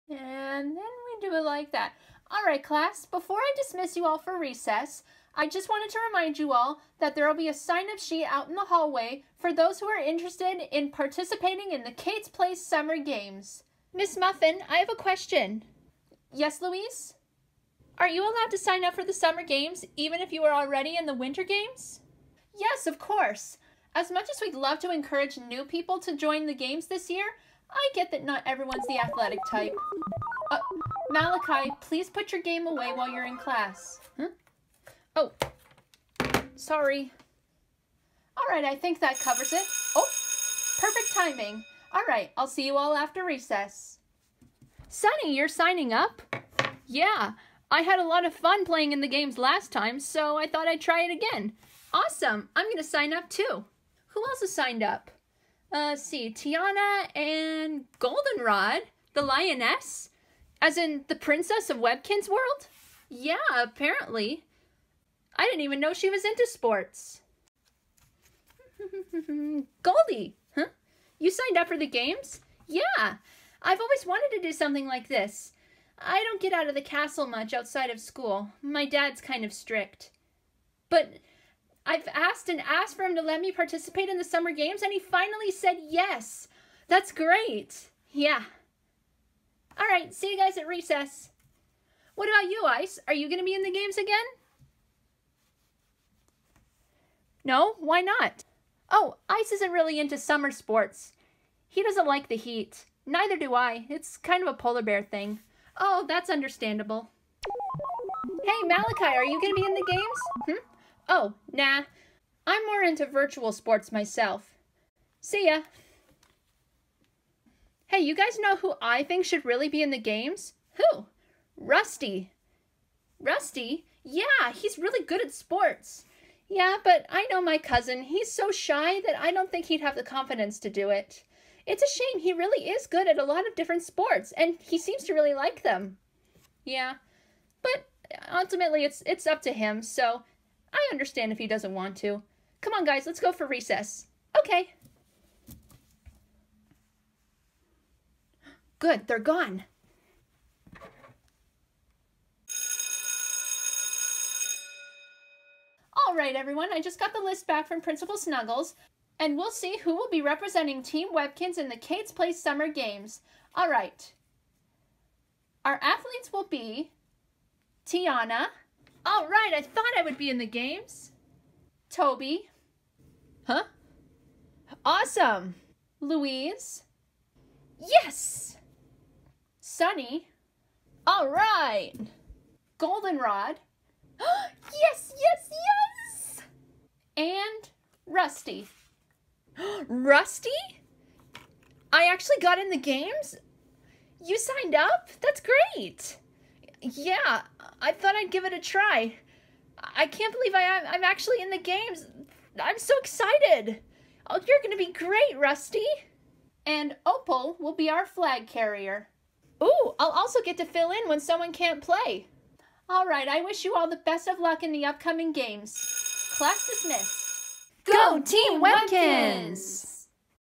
Team Webkinz
It's time to assemble the teams for the Kate's Place Summer Games! First up: Team Webkinz!